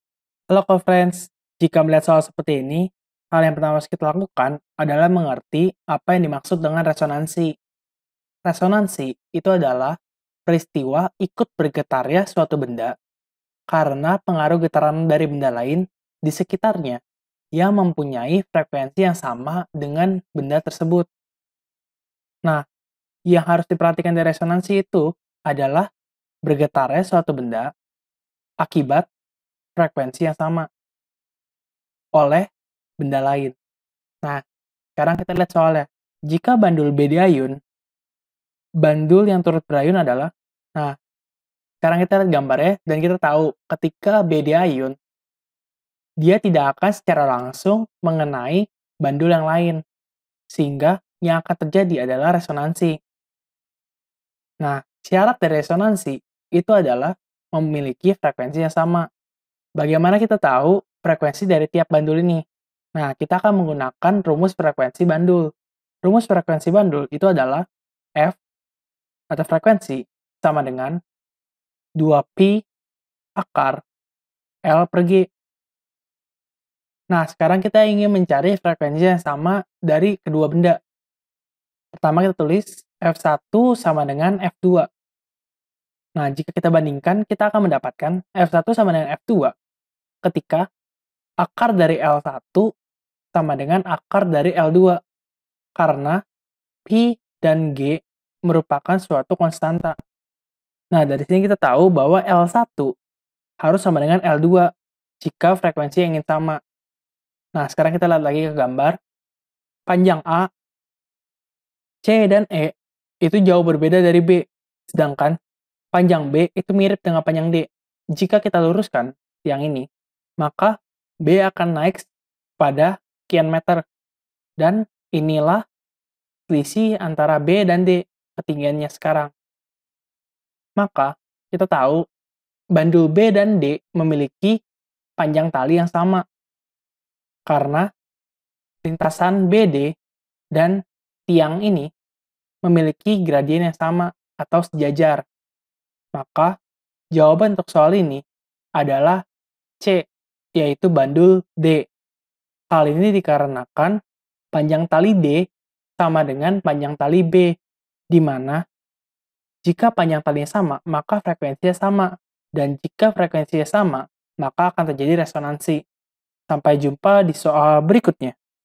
Perhatikan gambar berikut. B A E Jika bandul diayun, B bandul yang turut berayun adalah A D a_ C....
Perhatikan gambar berikut. B A E Jika bandul diayun, B bandul yang turut berayun adalah A D a_ C. b- C d. E

Topik atau Materi: Getaran (Amplitudo, Frekuensi, Periode) - Getaran dan Gelombang - Gelombang Mekanik - Fisika Kelas 8 SMP - K-13 Revisi

00:00 : Bedah Soal
01:00 : Konsep, rumus dan pengertian Getaran (Amplitudo, Frekuensi, Periode)
01:18 : Langkah penyelesaian soal
01:48 : Jawaban akhir
02:18 : Penutup